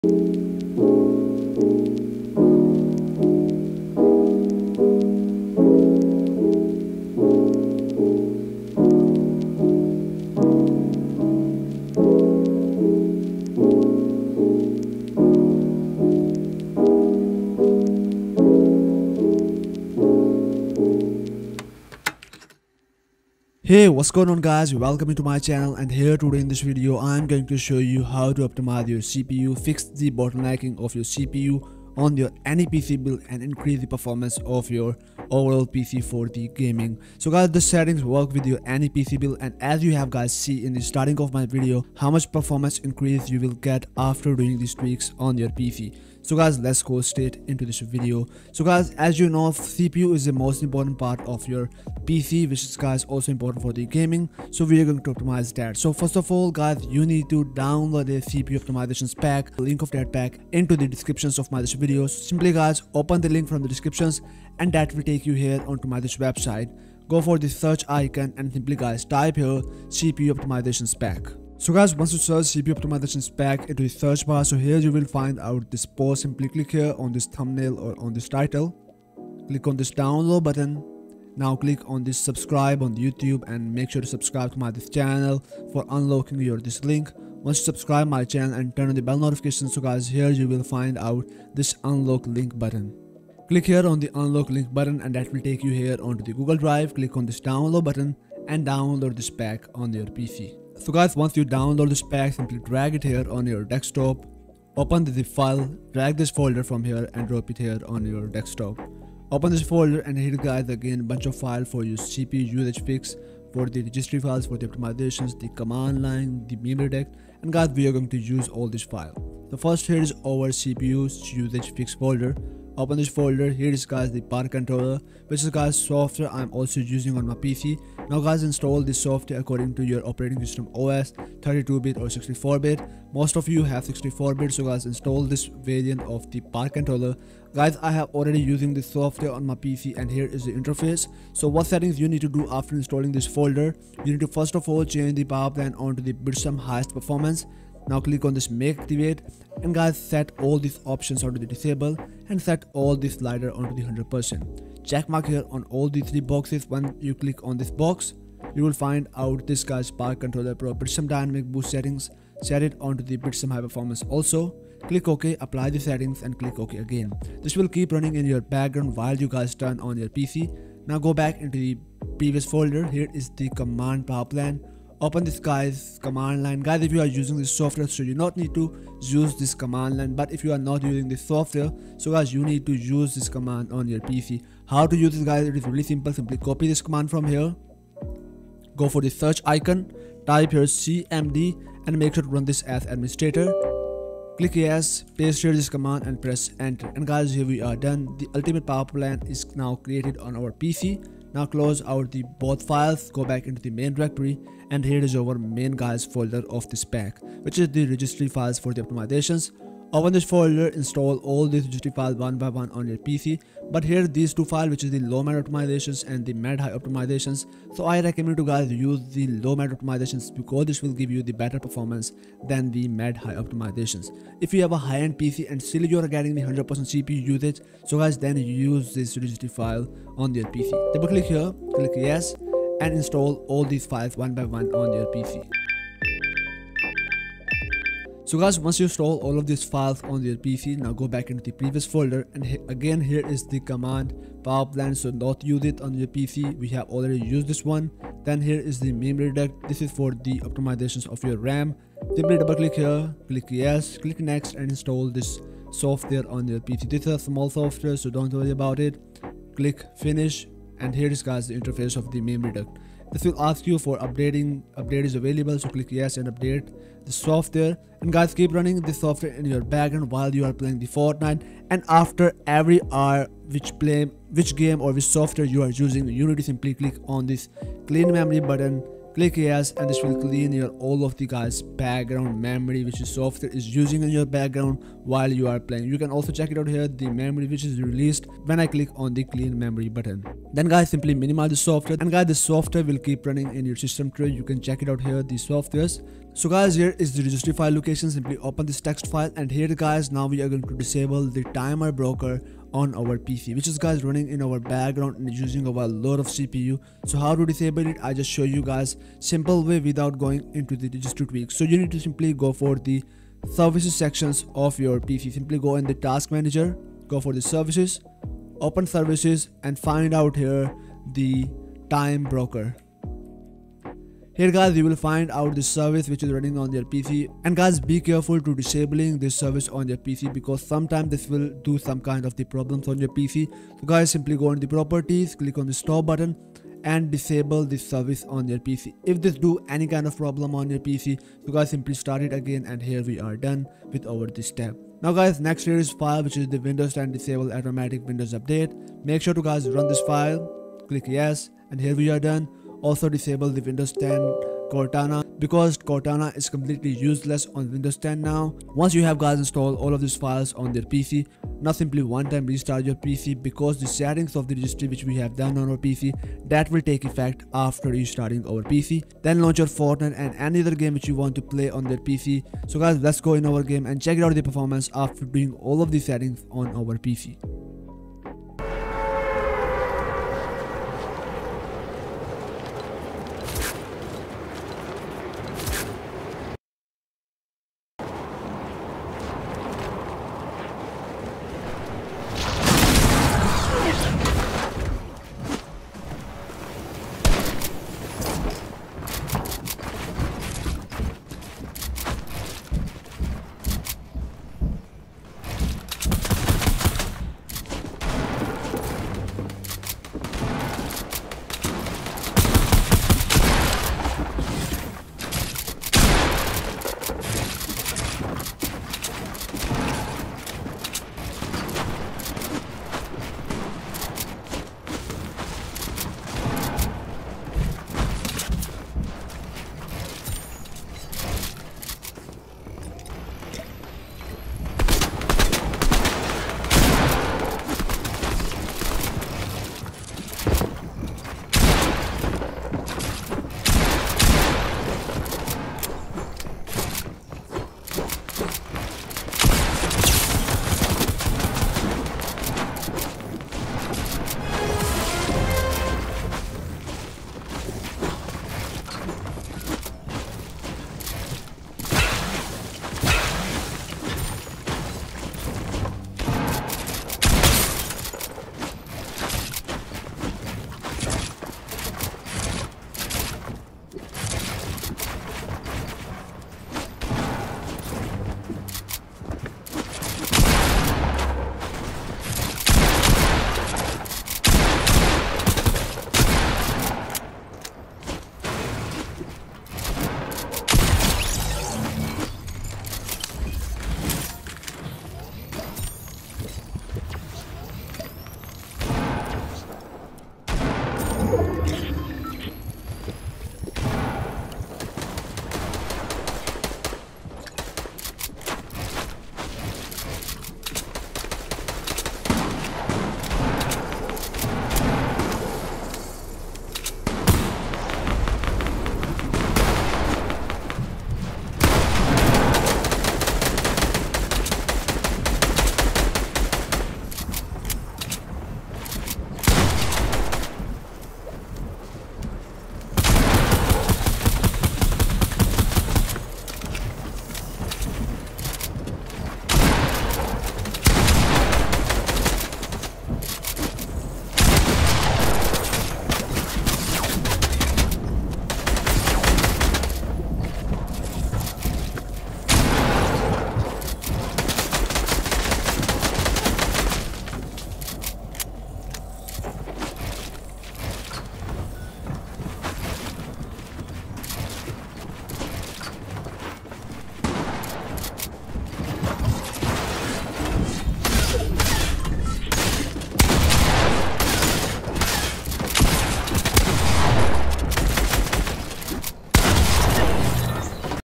How To Optimize CPU/Processor For Gaming 🔧 Fix FPS Drops & MAX FPS (2021)
Hello People!
Today in this video, I am showing you How To Optimize CPU/Processor For Gaming - Boost FPS & Fix Stutters Which Gives You the Best Performance For Any PC Build. It will also fix your High CPU Usage or 100% CPU Usage While You Gaming. So, If You Want To Boost Your FPS & Increase PC Performance So Make Sure To Watch Complete Video!

Title: How To Optimize CPU/Processor For Gaming 🔧 Fix FPS Drops & MAX FPS (2021)